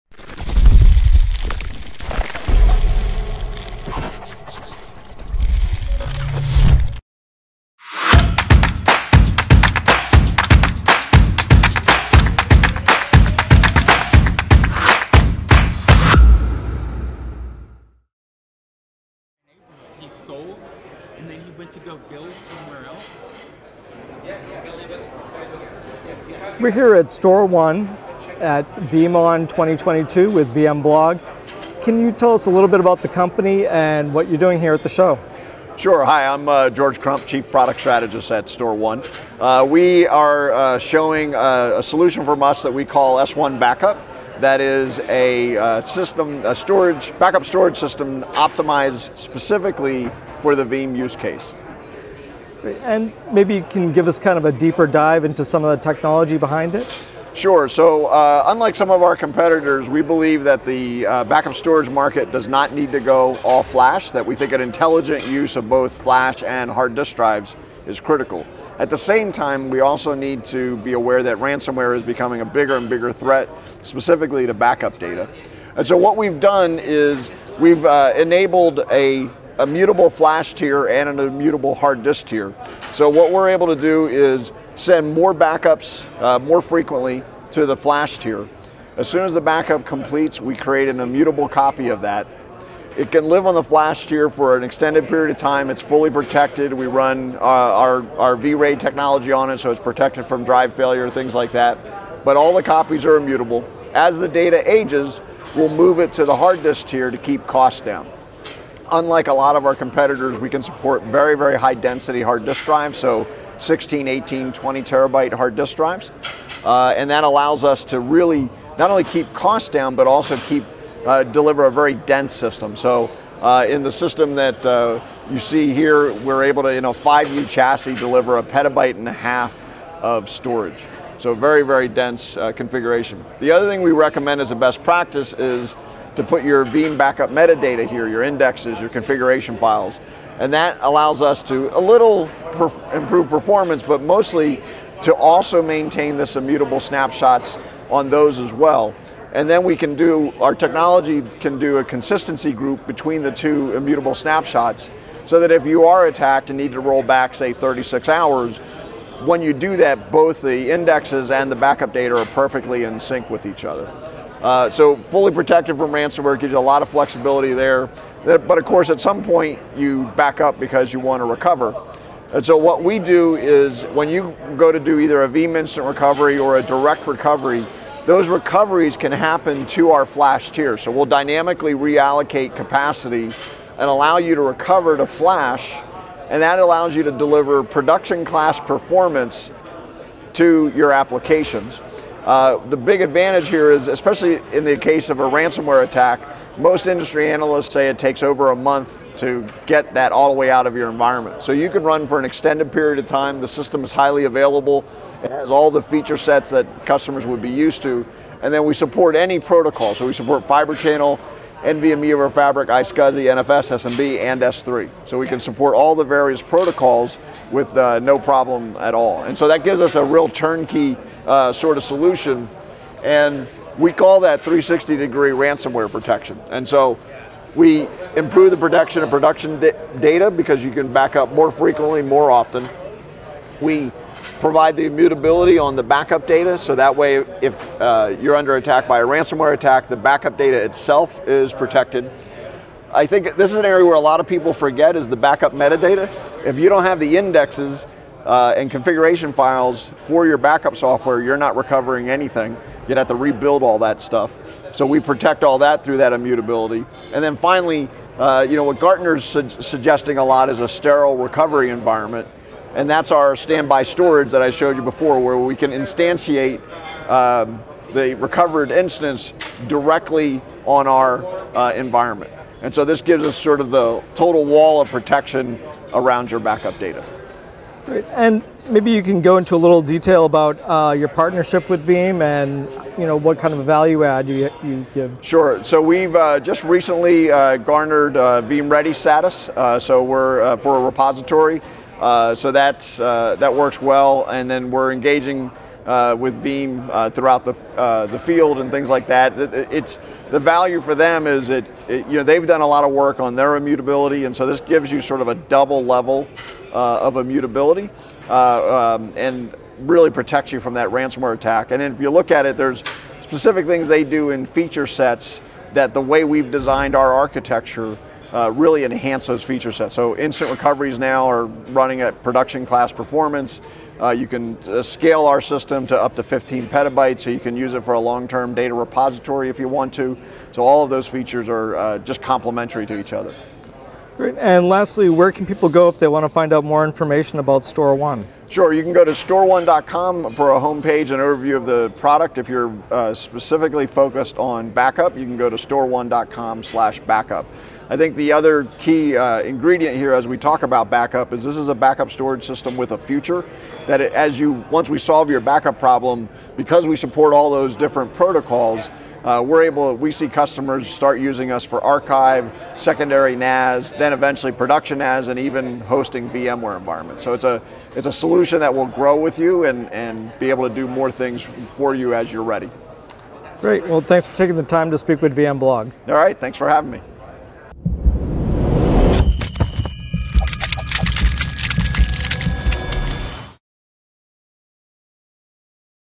StorONE at VeeamON 2022 - S1:Backup, 360° Ransomware Technology
VMblog visits with StorONE during VeeamON 2022 in Las Vegas to learn more about their offerings. StorONE is a storage software company, providing a practical storage consolidation strategy that mitigates risk and eliminates compromise.

At the event, StorONE is talking about their S1:Backup which raises the bar for backup storage, enabling Veeam to reach its full potential. Their flash-first technology enables you to improve RPO and RTO, while their intelligent use of hard disk drives makes long-term data retention more affordable than ever. Combine that with their instant immutability, and you have the foundation of a solid ransomware recovery strategy.

Watch and learn more from our conversation at VeeamON.